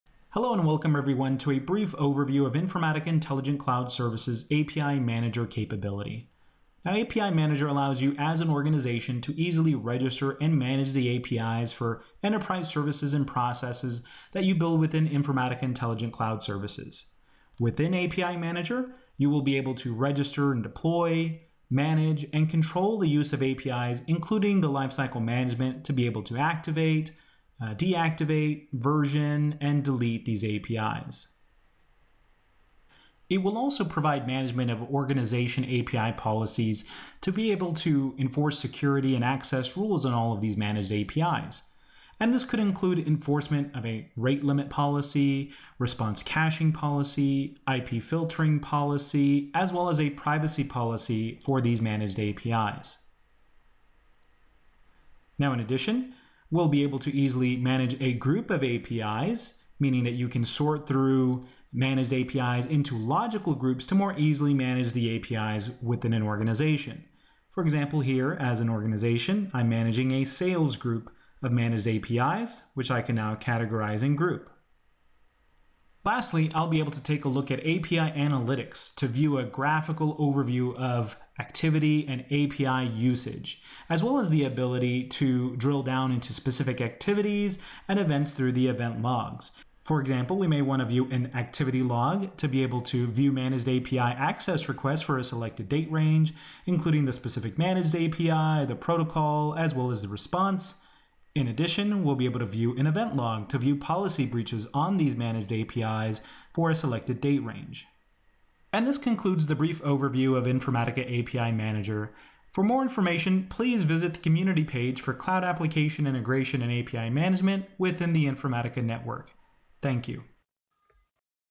Two Minutes Overview of the Informatica API Manager
This video provides brief capability overview of API manager available as part of Informatica Intelligent Cloud Services.